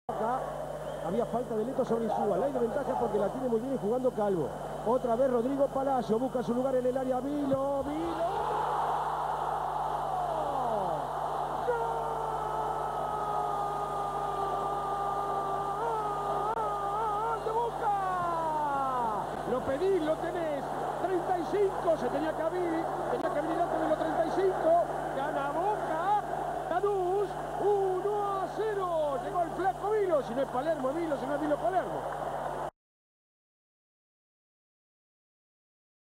Gol de Bilos a Lanus (Boca 1-Lanus 1 21-08-05)
SI TE GUSTÓ EL VIDEO, PODÉS COLABORAR CON UNA DONACIÓN

Gol de Bilos a Lanus a los 35' por el Torneo Apertura 2005 (Boca 1-Lanus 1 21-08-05)